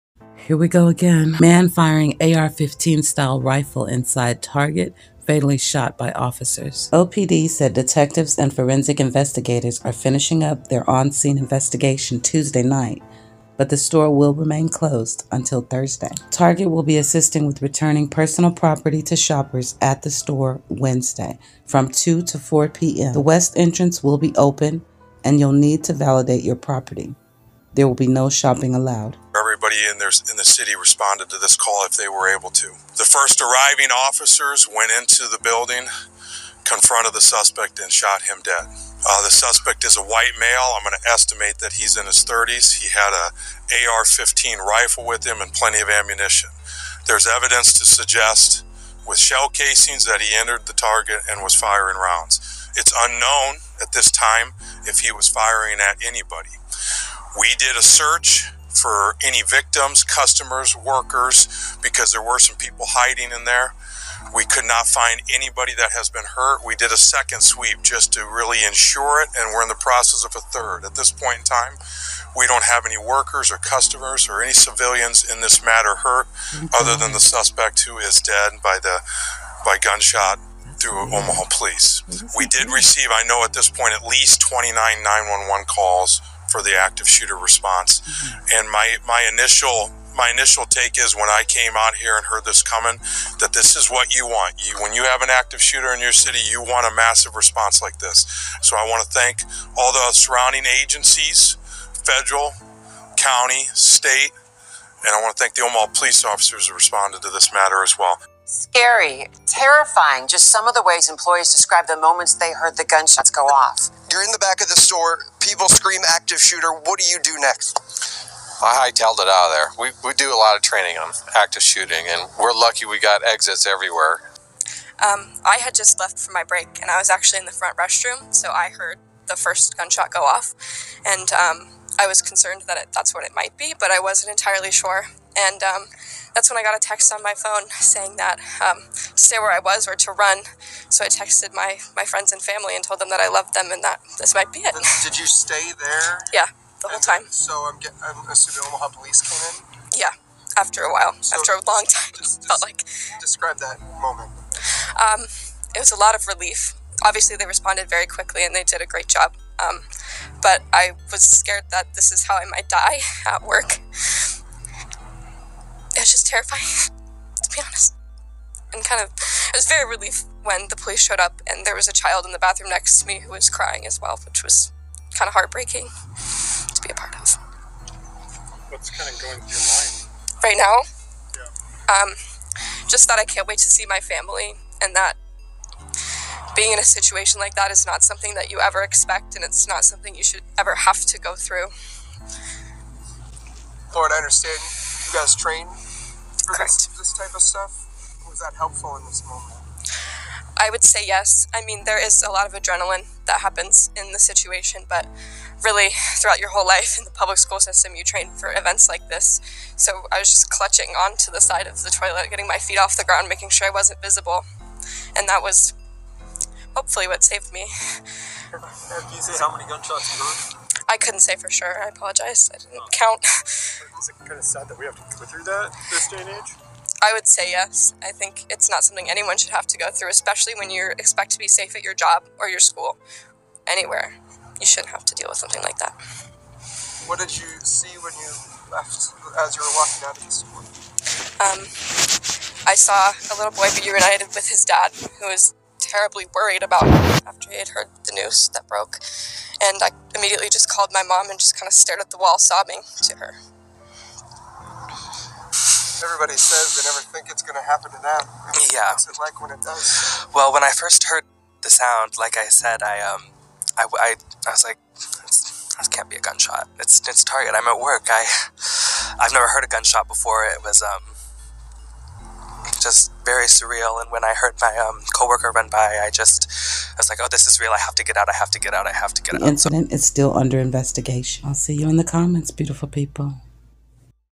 The Nebraska Target Shooting: What REALLY Happened?
No customers or workers were shot in the incident. The gunman, who was shot and killed by police, had an AR-15 style rifle, officials said.
🎶 by Slikk Mouth Records